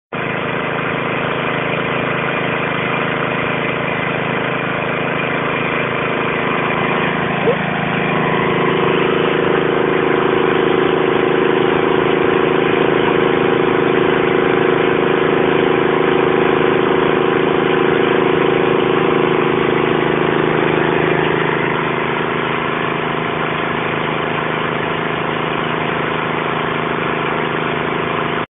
Ryan Ferguson of Ruby Painting in action
Power washing the house Looked like fun to me!